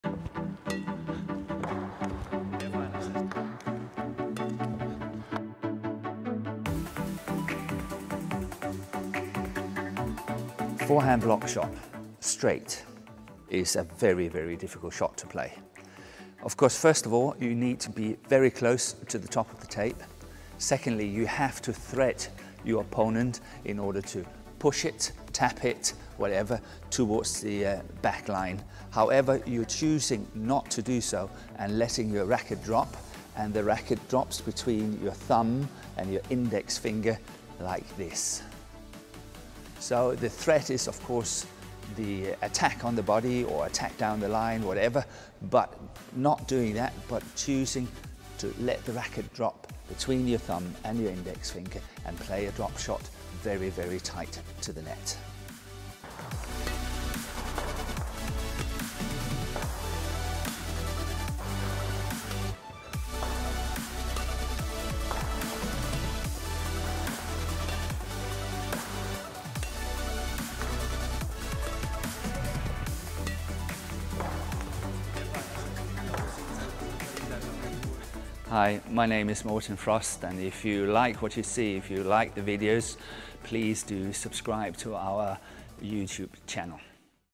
Forehand net block shot | By Basic Feather & Morten Frost | Badminton tutorial
Watch the badminton tutorial of the forehand net block shot. The video will give you an alternative to the push at the net, being able to make your opponent more hesitant on what shot you will play.

1. Get the shuttle early
2. Let the racket drop in your fingers
3. Practice, practice, practice!

A bonus info on this shot, Lee Chong Wei loved to do it in his forehand. When he rushed to the net, but chose to play the block shot instead of the drive.

🏸The new badminton equipment used and trusted by Morten Frost, Basic Feather